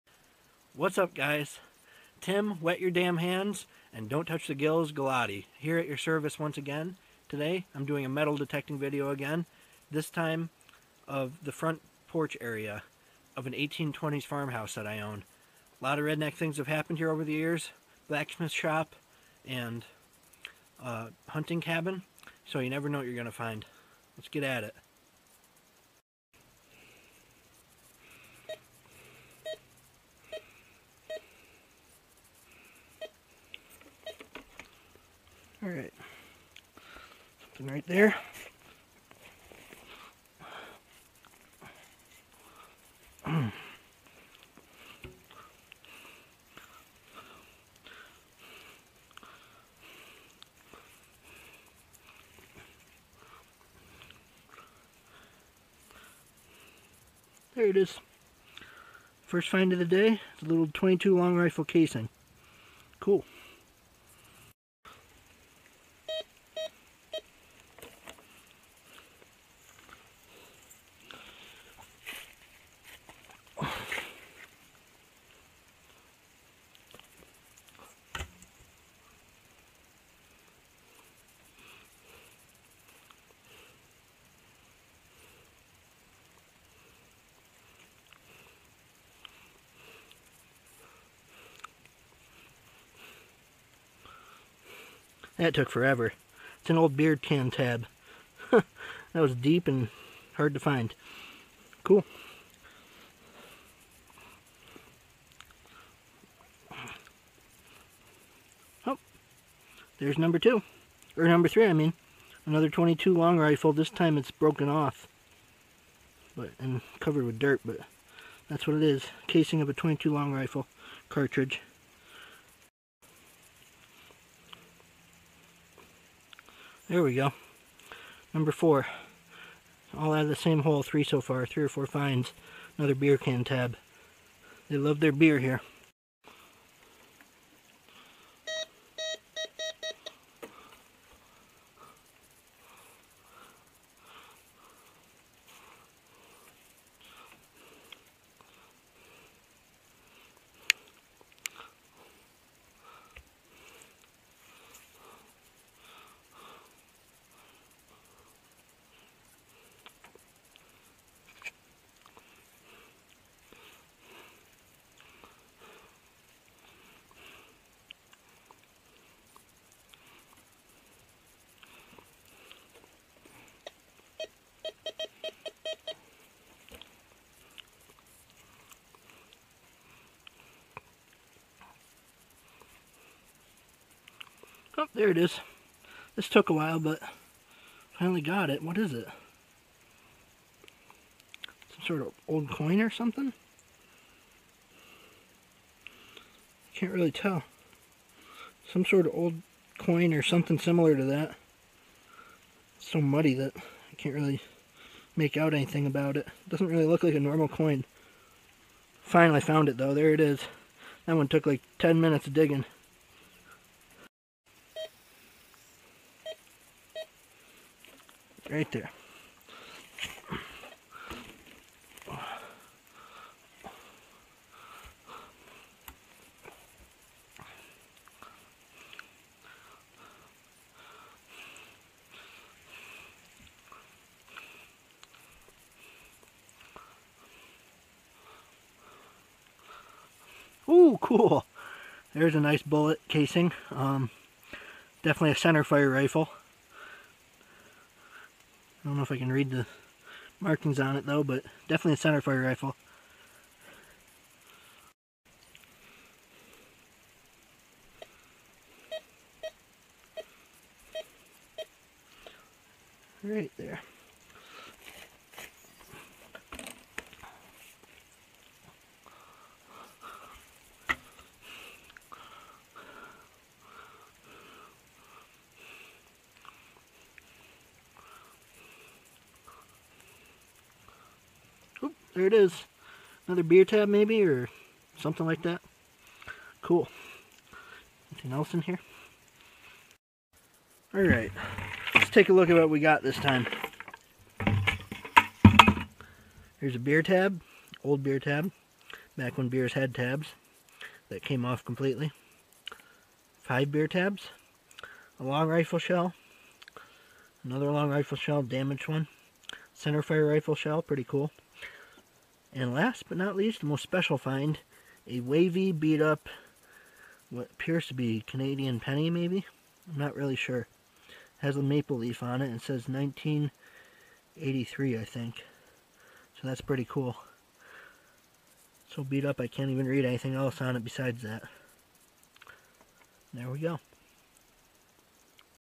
Vlog #78: Metal Detecting at 1820s Cottage | Bullet Casings, Beer Tabs, and a Coin! (P2)
This is my seventy eighth vlog (video blog) filmed in July, 2018. It shows an evening of metal detecting (treasure hunting) around the front porch area of an 1820s wooded upstate New York farmhouse that has served as a blacksmith shop and hunting camp for the last two centuries. During this session, I found beer tabs, bullet casings, and a coin.